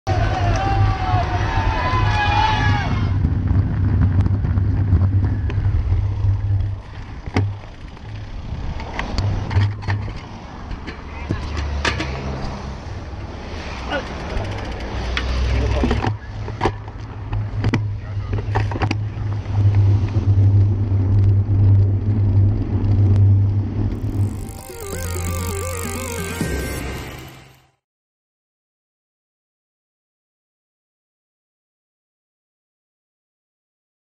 Tour de France 2017 | Stage 6 Moment (Wheel Change)
GoPro footage from the peloton as Jurgen Roelandts has a fast wheel change during Tour de France Stage 6.